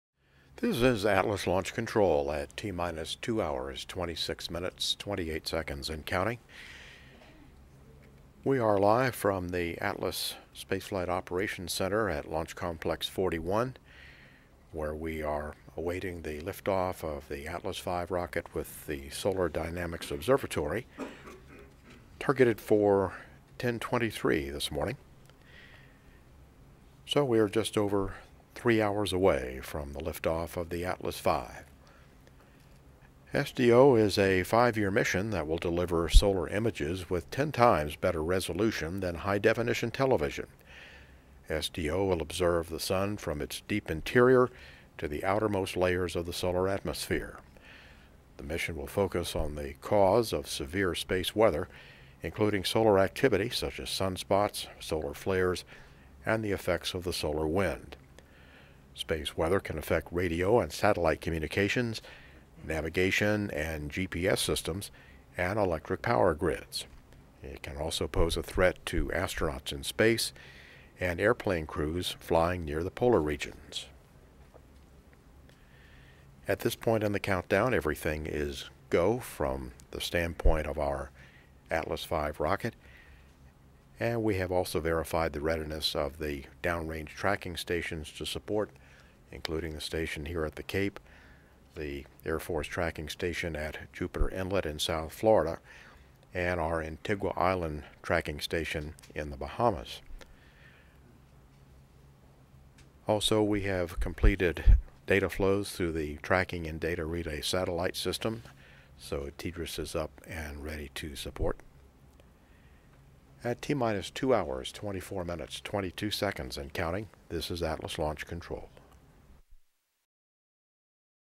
Introduction to SDO Launch Commentary
NASA Commentator George Diller opens countdown coverage for the Atlas V launch of the Solar Dynamics Observatory.